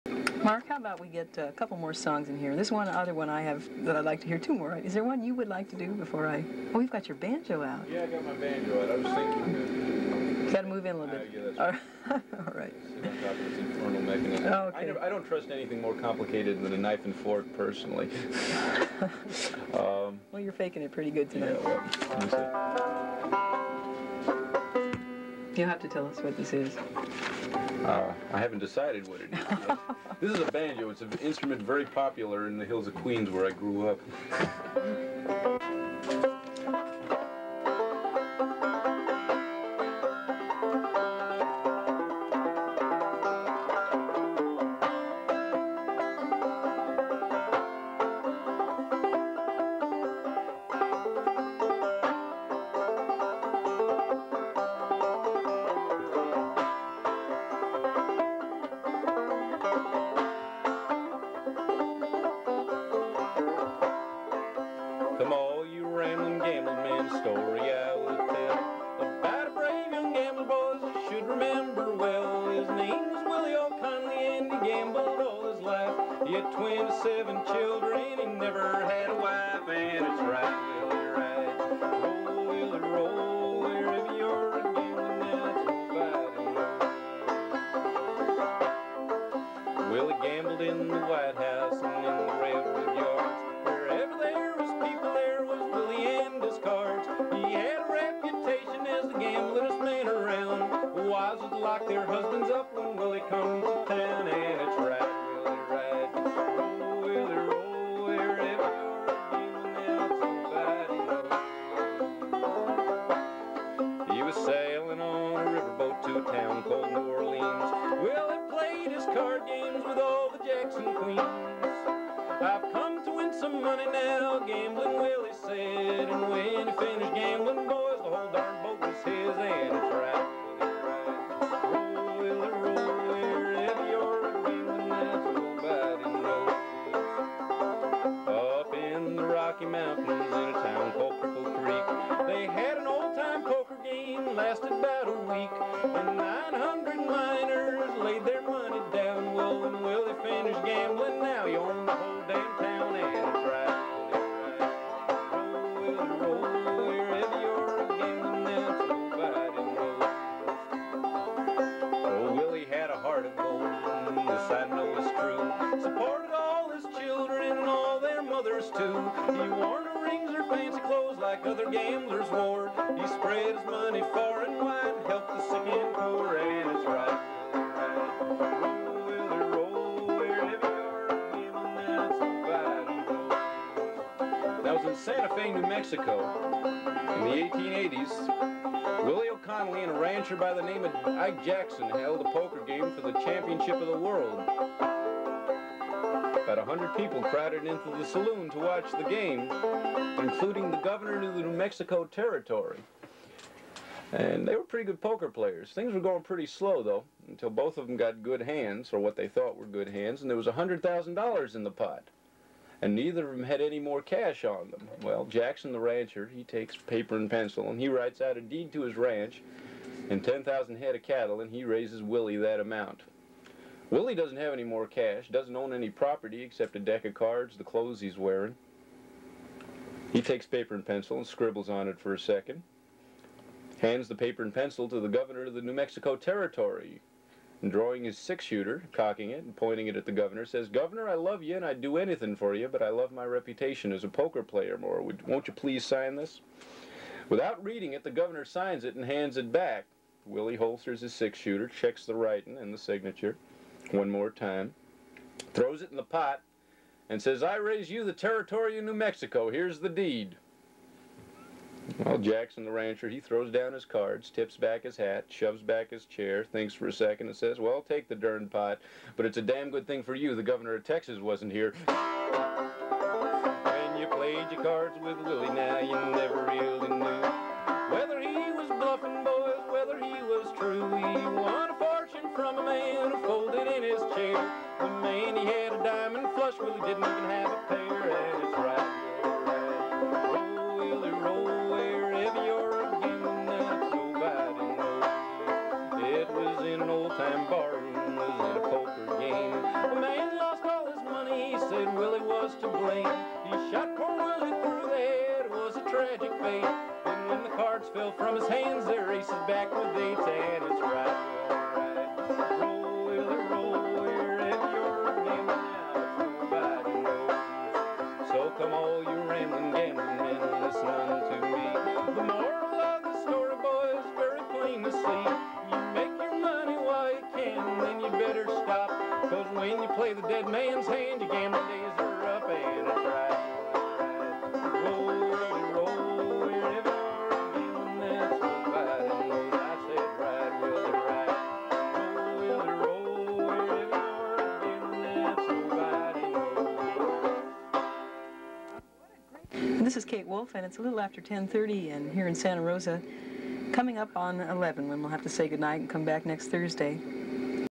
Kate Wolf with Mark Ross
Kate interviewed Mark Ross on her radio show. He plays a banjo tune about a gamblin man.